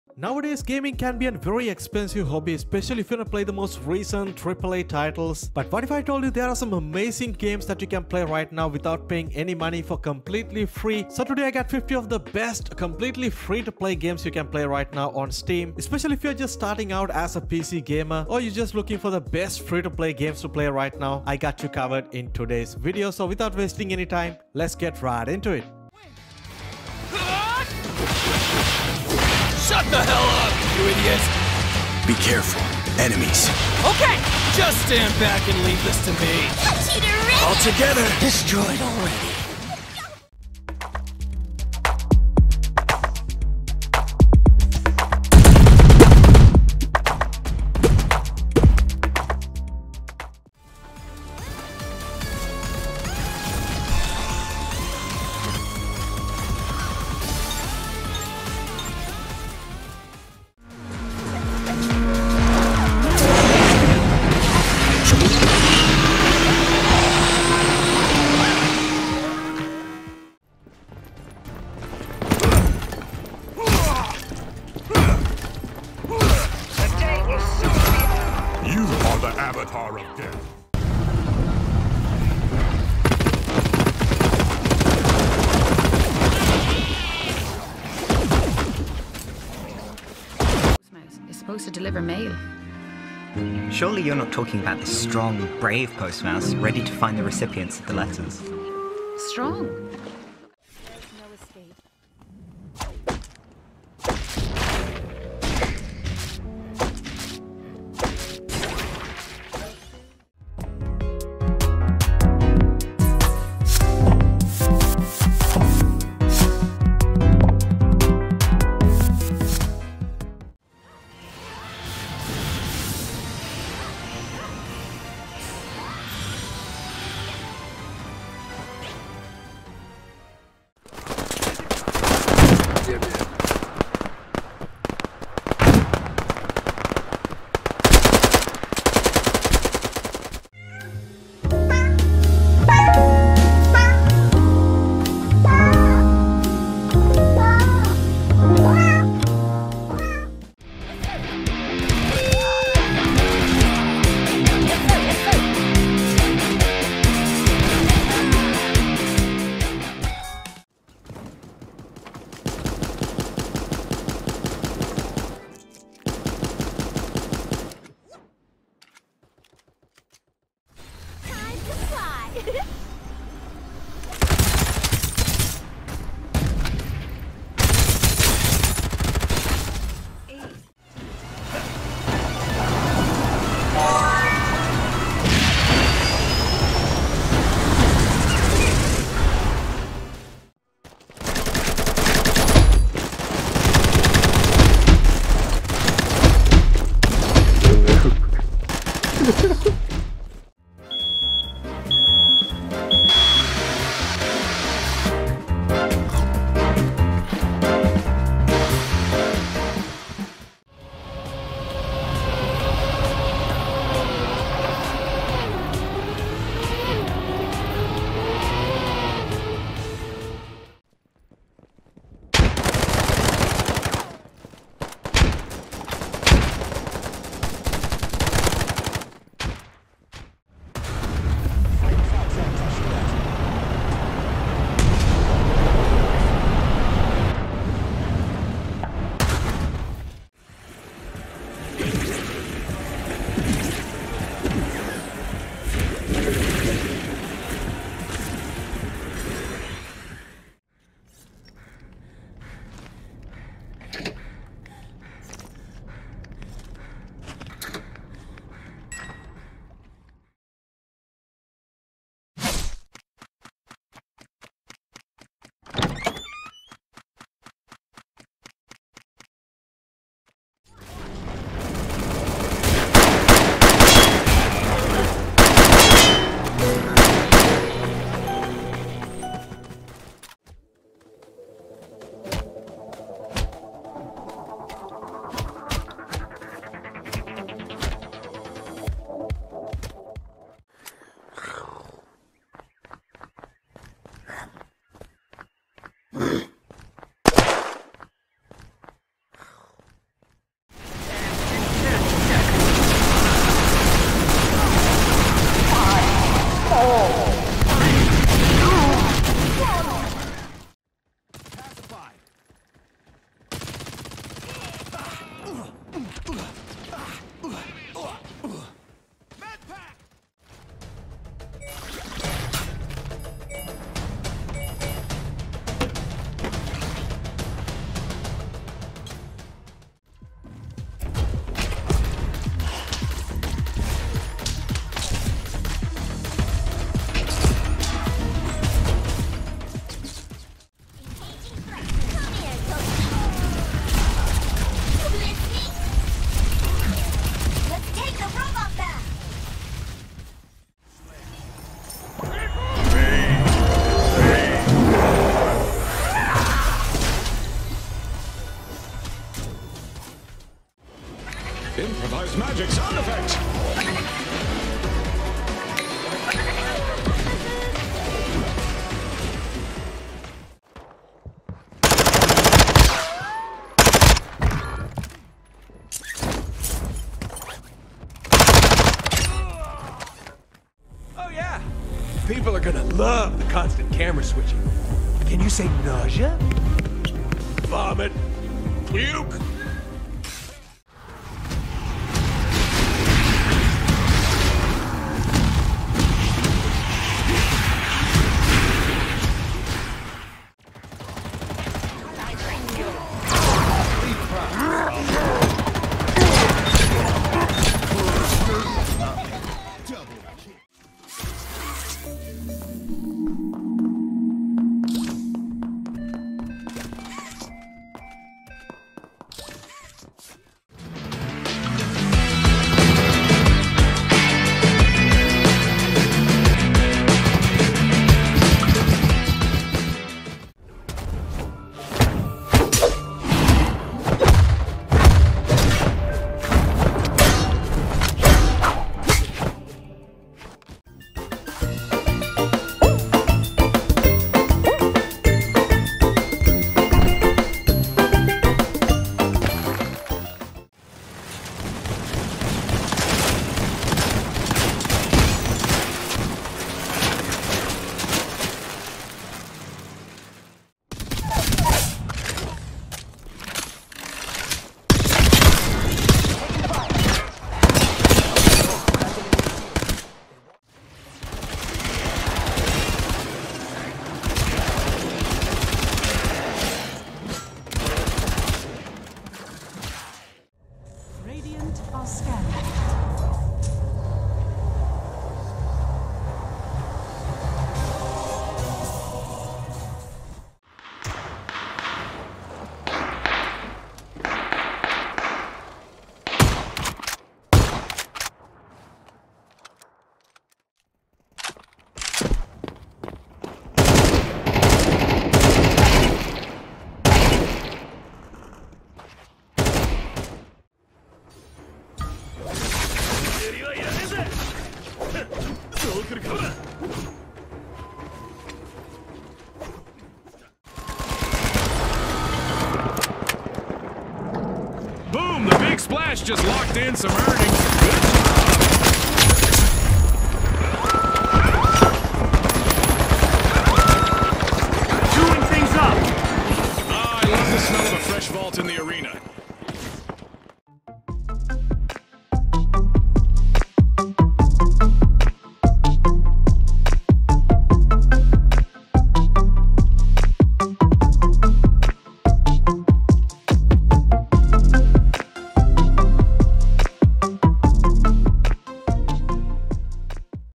Top 50 FREE Steam Games to play in Early 2024!🔥
Hey guys!😊
Today we are checking out TOP 50 New Free-to-Play Steam GAMES you should play in 2024. You can easily download them through Steam, and they all free to play.